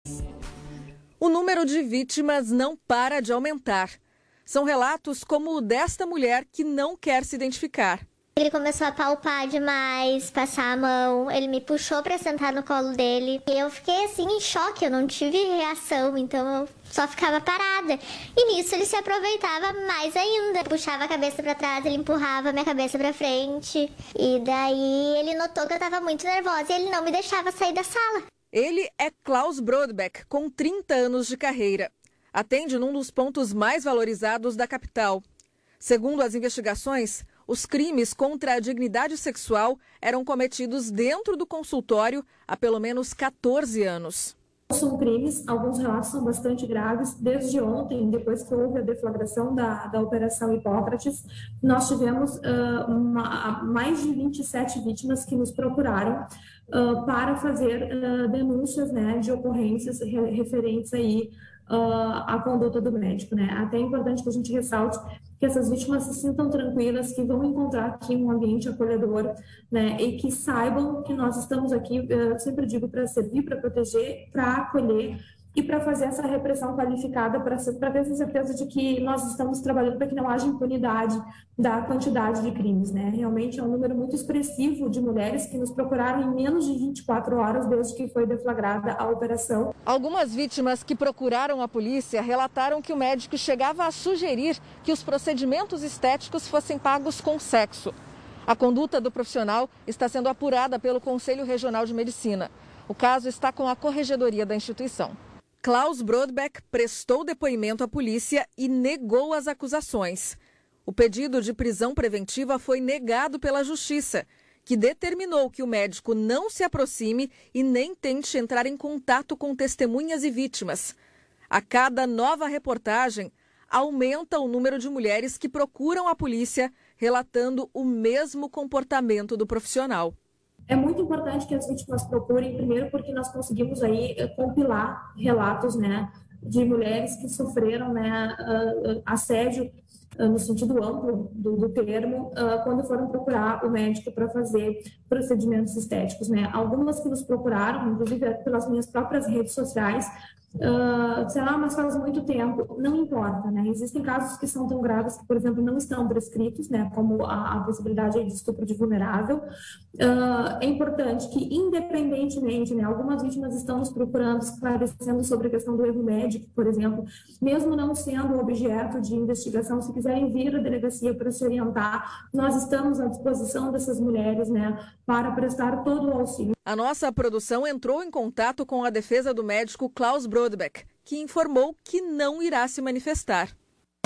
Abuso sexual: novas vítimas de médico procuram a polícia
Até agora quase 40 mulheres procuraram a polícia para denunciar abusos sexuais praticados por um cirurgião plástico da capital. A polícia pediu a prisão preventiva dele, mas a justiça negou.